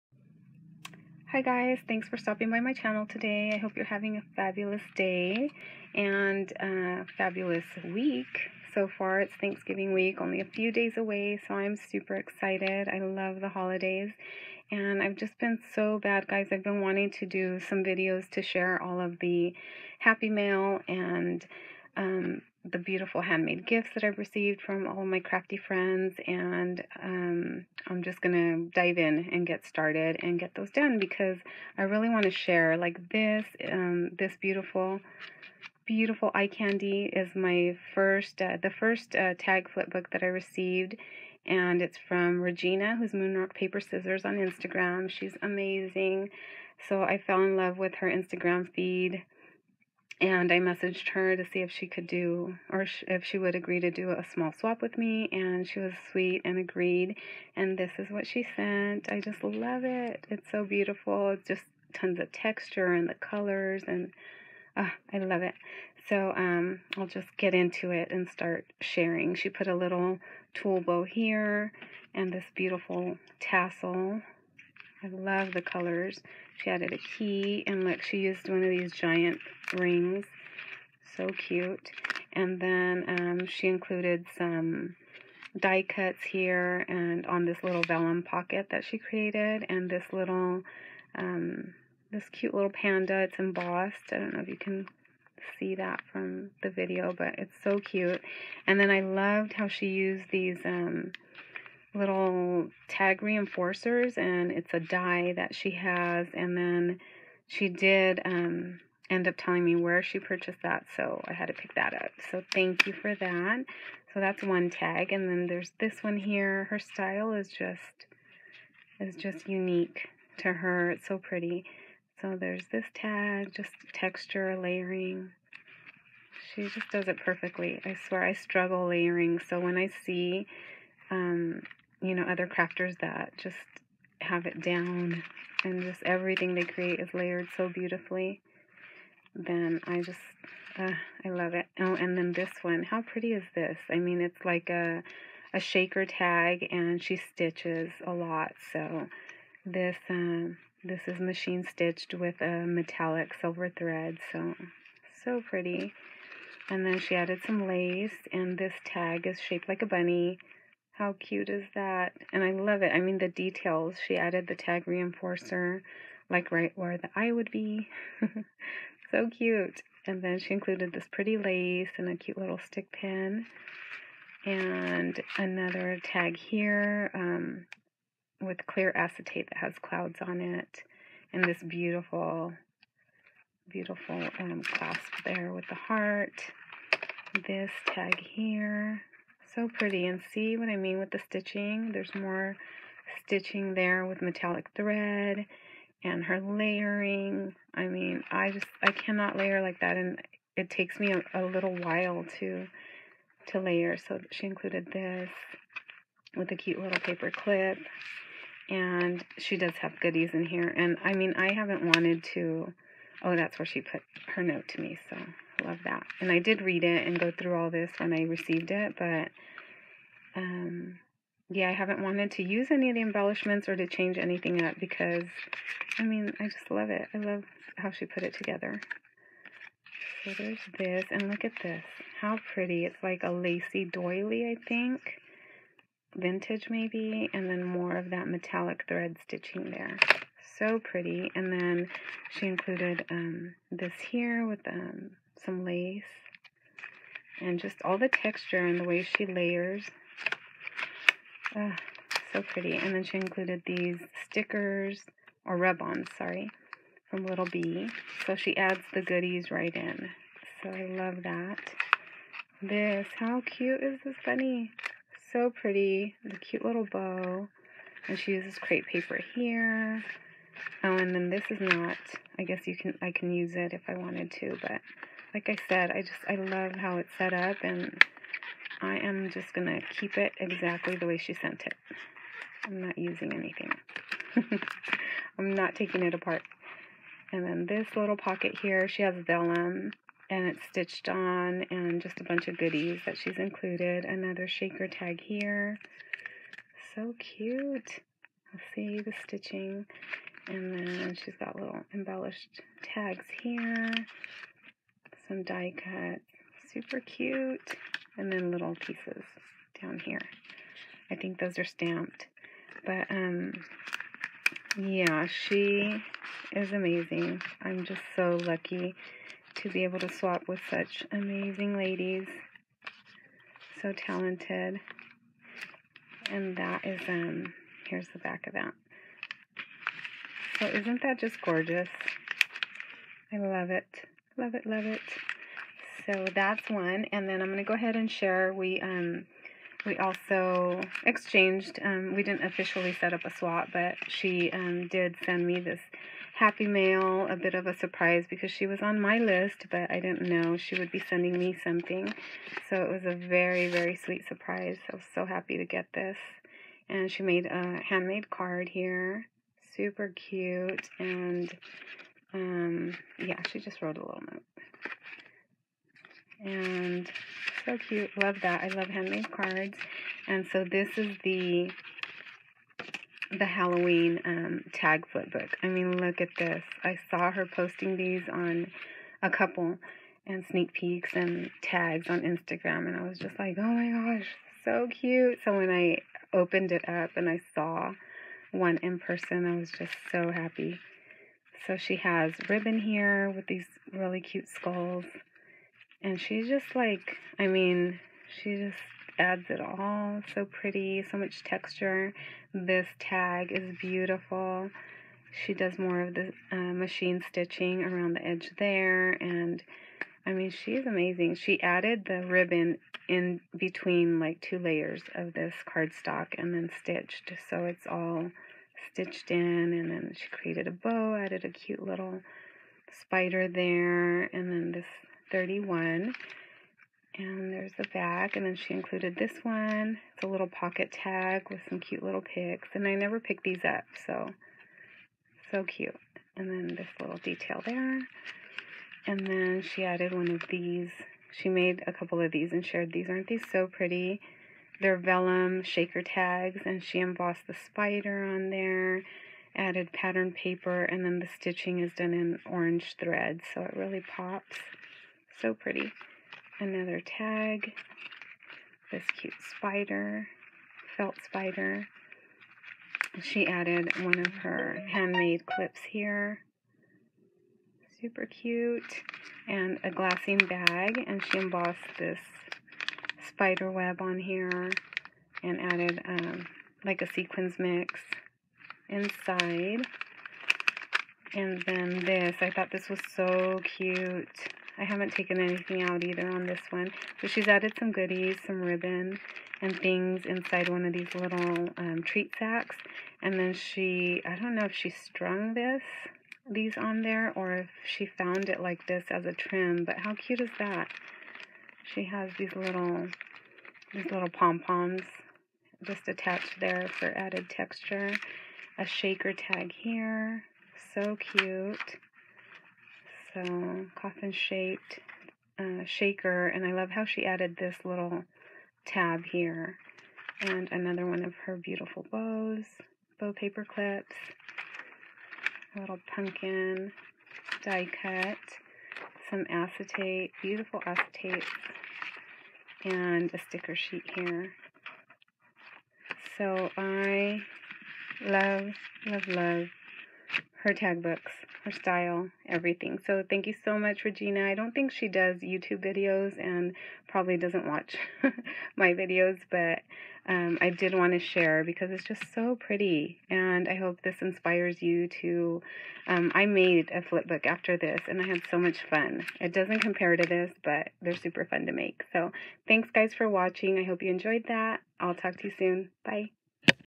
Eye Candy! Happymail Share
Hi Ladies! I am so behind on sharing all the beautiful mail I have been so lucky to receive in the last few months! These amazing flipbooks were created by Regina, IG: @moonrock.paper.scissors. I hope you enjoy taking a look at her creations, I love every detail!! Check her out on Instagram : )